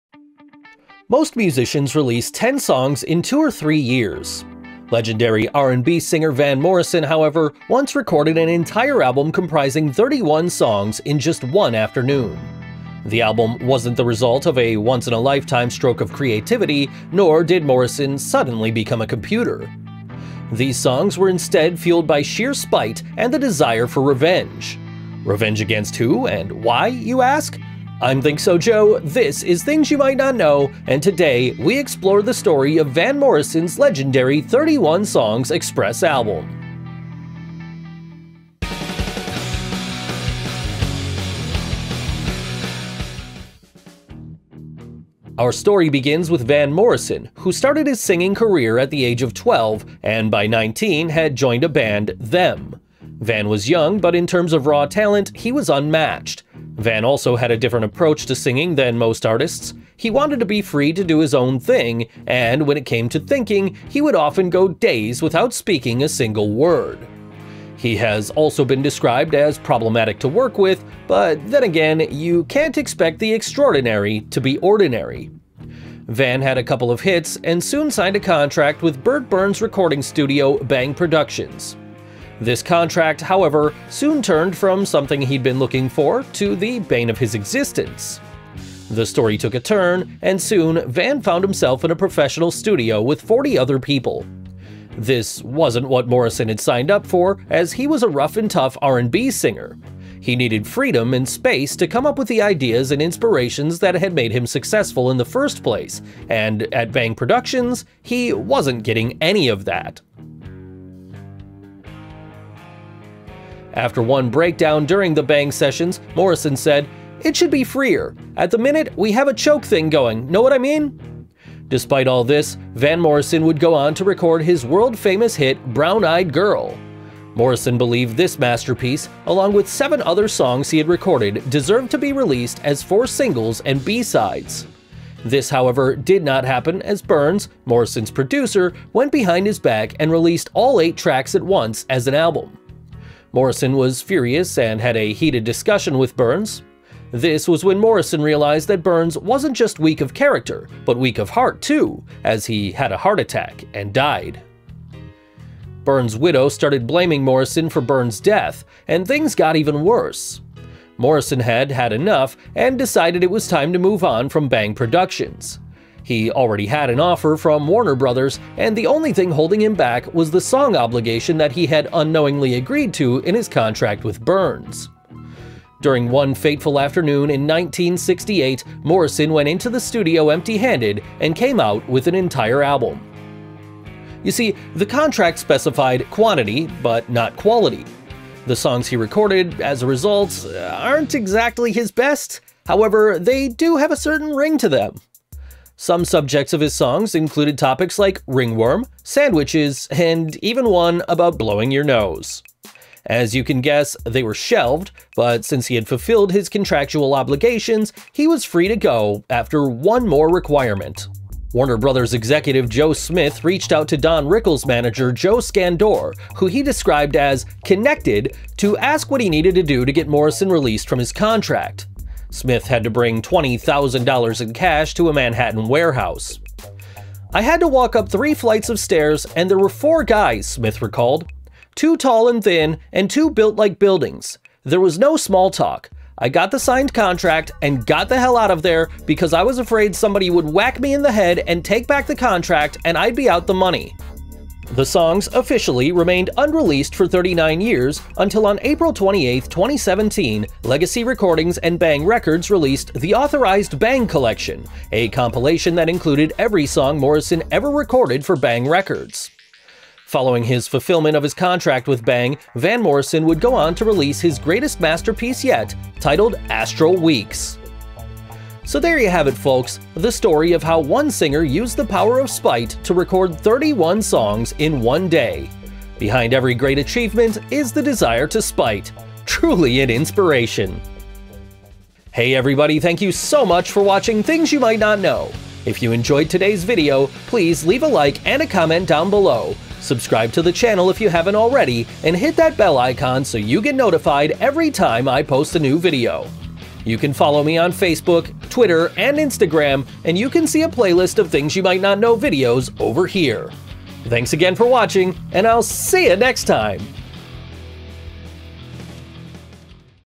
Van Morrison's 31 song "screw you" to Bang Records - Things You Might Not Know
In 1968, in an effort to fulfill his contractual obligations to Bang Records, the legendary Van Morrison walked into a studio and crapped out an album of questionable quality off the top of his head.  Today on "Things You Might Not Know," it's Van Morrison's 31 song "screw you" to Bang Records.